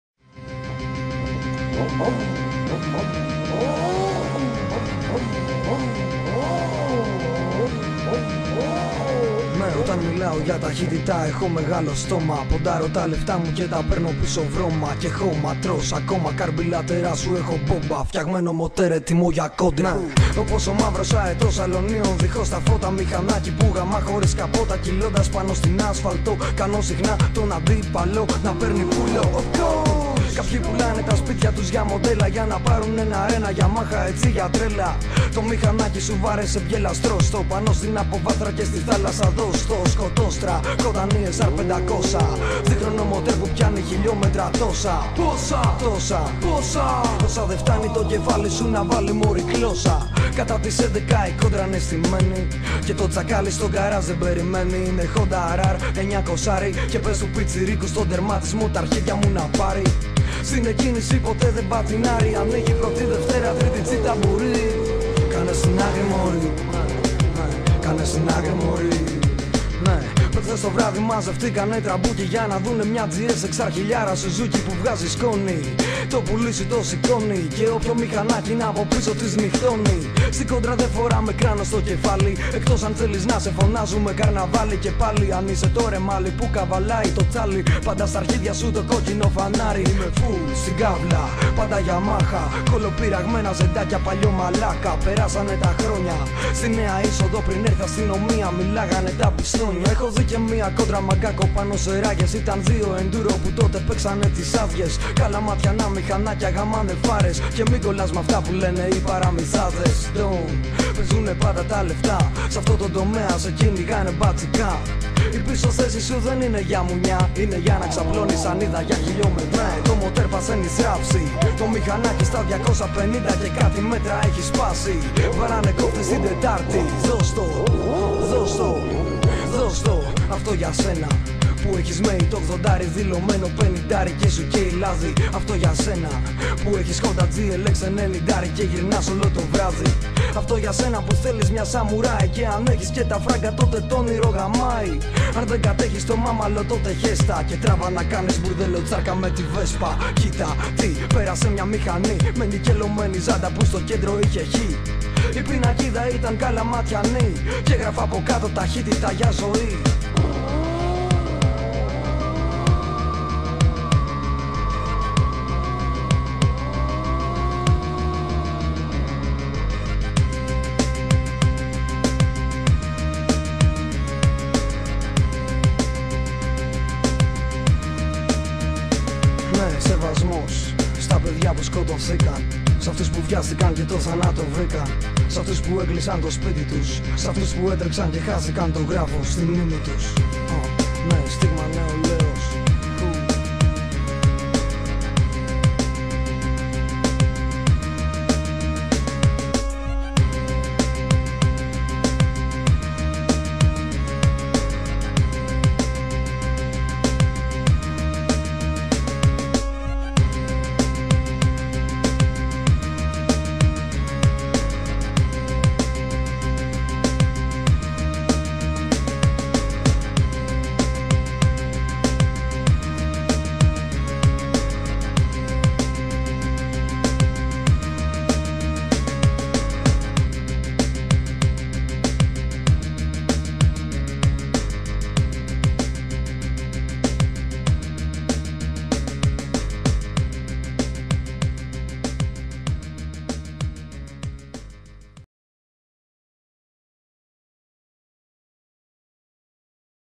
taxitita gia zwh
akouste thn mousikh k apolafste thn mixanh ka8e peripou 1 lepto alazei k h foto